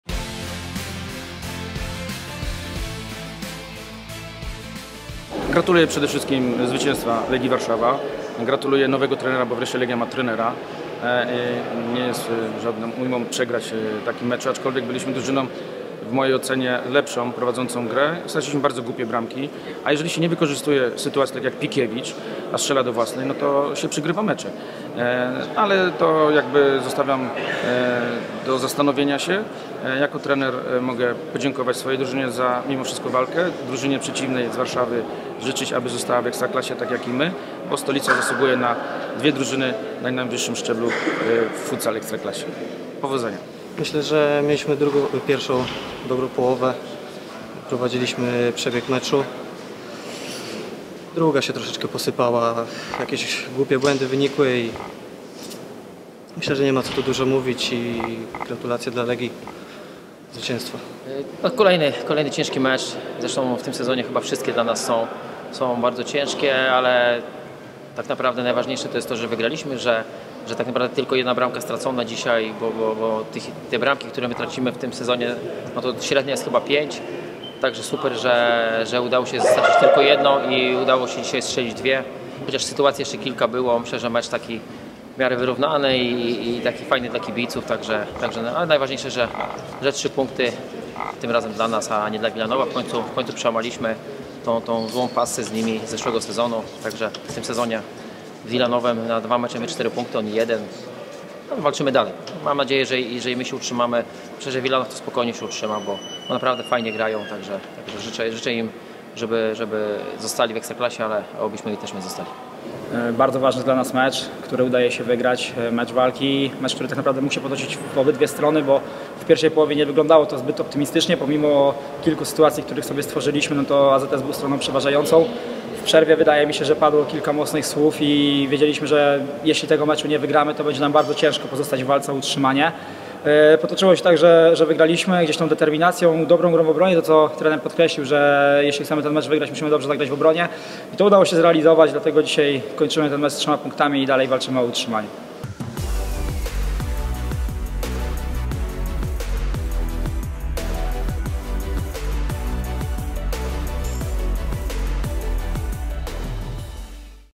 AZS UW DARKOMP - Legia Warszawa 1:2 | WYPOWIEDZI | 21. kolejka (2022/23)
AZS UW DARKOMP Wilanów vs. Legia Warszawa 1:2 (1:0) | 21. kolejka - sezon 2022/23

BRAMKI: Grzegorz Och 4 - Mariusz Milewski 24, Maciej Pikiewicz 31 (s).

Po meczu wypowiedzieli się trenerzy Maciej Karczyński i Mariusz Milewski oraz Marcin Mianowicz i Tomasz Warszawski.

Oglądaj MECZE KOLEJKI w każdy poniedziałek o godzinie 18:15 w TVP Sport!